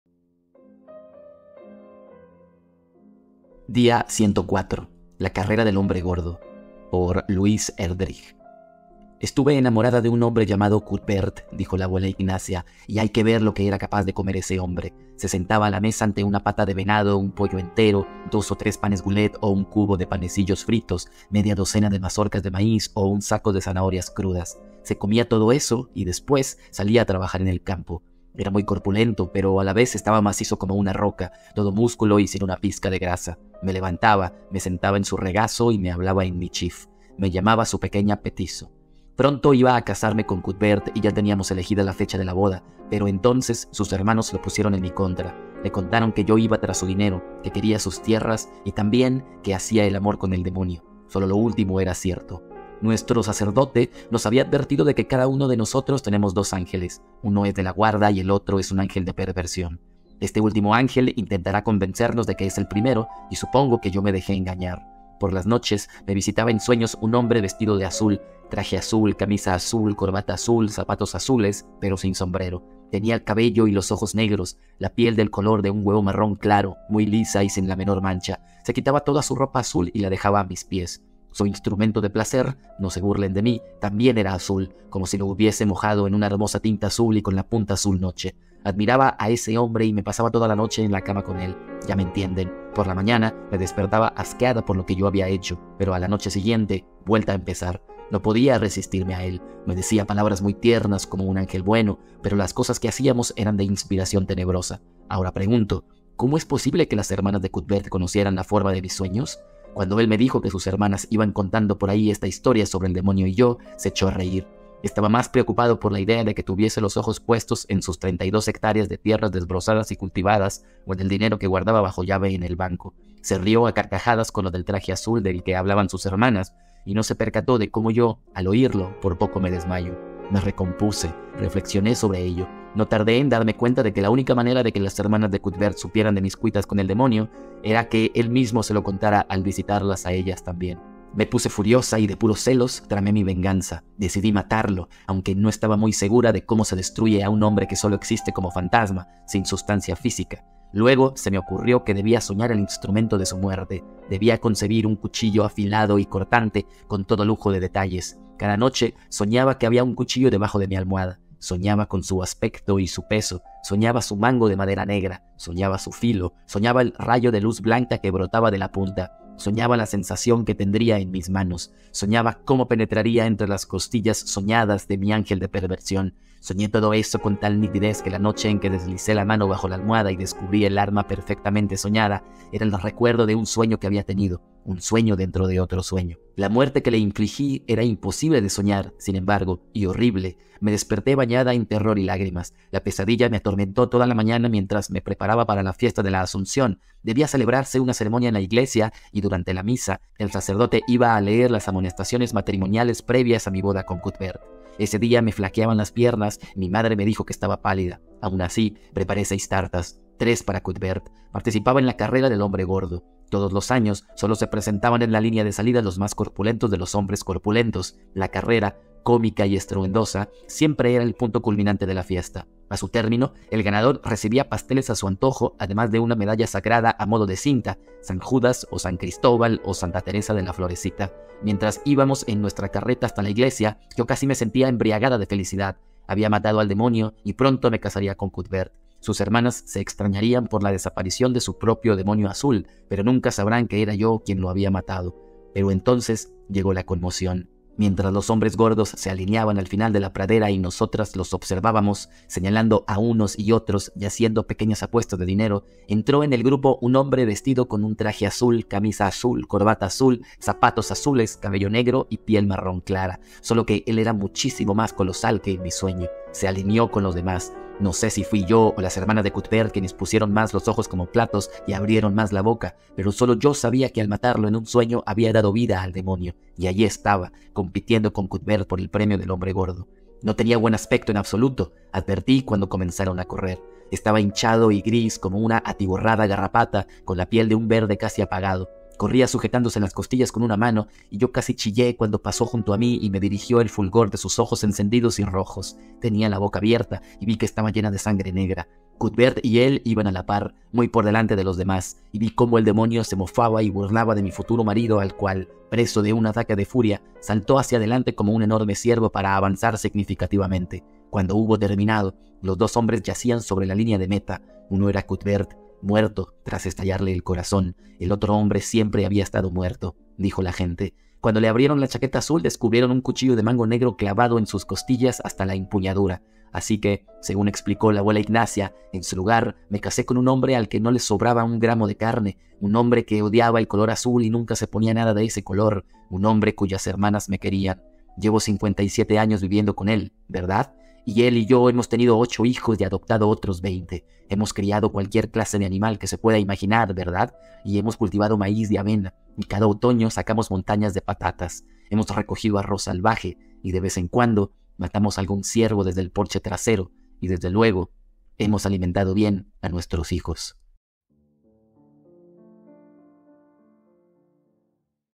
Día 104 - La carrera del hombre gordo, por Louise Erdrich
Literatura todos los días: un cuento cada 24 horas.

Hoy, día 104 - La carrera del hombre gordo, por Louise Erdrich

Gracias por escucharme. ¡Abrazo!